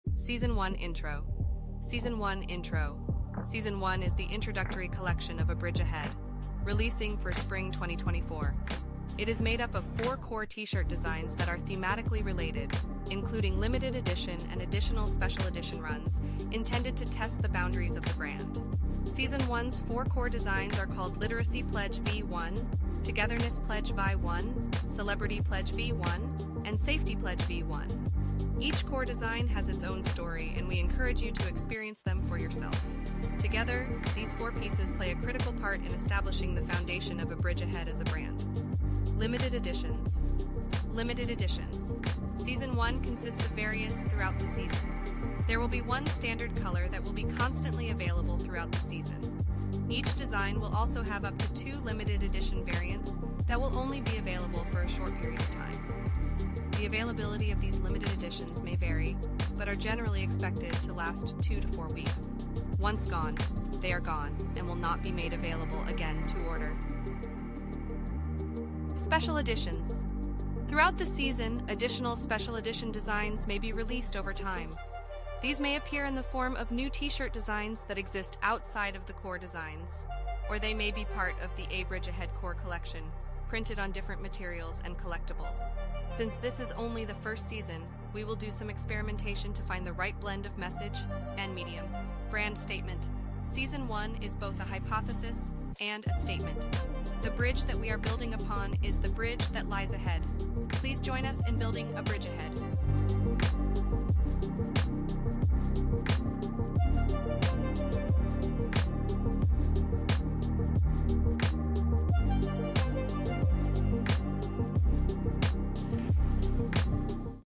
A Bridge Ahead Season One Trailer v1
A Bridge Ahead is a new fashion brand. Its first offerings are now for sale

Transcript:
Season One Intro:
Season One is the introductory collection of A Bridge Ahead, releasing for Spring 2024. It is made up of four core T-Shirt designs that are thematically related, including limited edition and additional special edition runs, intended to test the boundaries of the brand.

Season One’s four core designs are called Literacy Pledge v1, Togetherness Pledge v1, Celebrity Pledge v1, and Safety Pledge v1. Each core design has its own story and we encourage you to experience them for yourself. Together, these four pieces play a critical part in establishing the foundation of A Bridge Ahead as a brand.

Limited Editions:
Season One consists of variants throughout the season. There will be one standard color that will be constantly available throughout the season. Each design will also have up to two limited edition variants that will only be available for a short period of time. The availability of these limited editions may vary but are generally expected to last 2-4 weeks. Once gone, they are gone, and will not be made available again to order.

Special Editions:
Throughout the season, additional special edition designs may be released over time. These may appear in the form of new T-Shirt designs that exist outside of the core designs, or they may be part of the A Bridge Ahead core collection, printed on different materials and collectibles. Since this is only the first season, we will do some experimentation to find the right blend of message and medium.

Brand Statement:
Season One is both a hypothesis and a statement. The Bridge that we are building upon is the Bridge that lies Ahead. Please join us in building A Bridge Ahead.